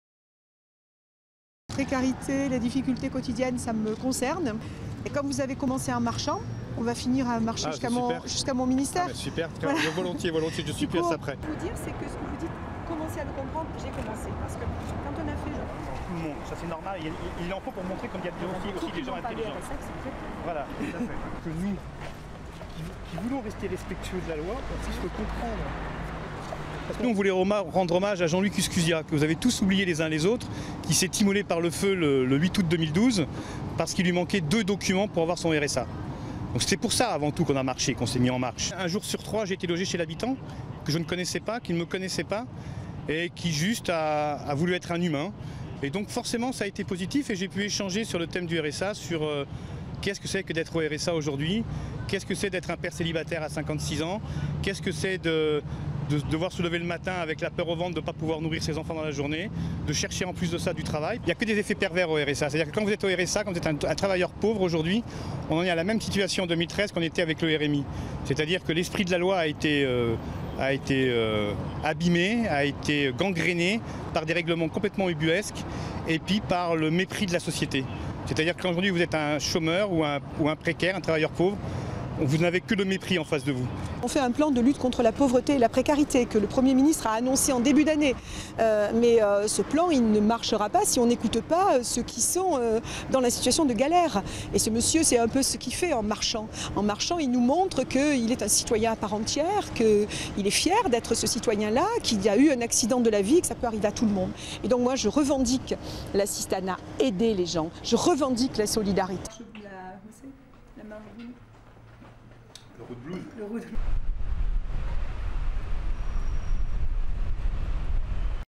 RSA: Alain Guézou rencontre Carlotti après une marche de 612 km
Alain Guézou, a rencontré mercredi 28 août la ministre déléguée à la lutte contre l'exclusion Marie-Arlette Carlotti, après avoir parcouru à pied les 612 kilomètres entre Grenoble et Paris, pour dénoncer les effets "pervers" du RSA.Durée: 01:58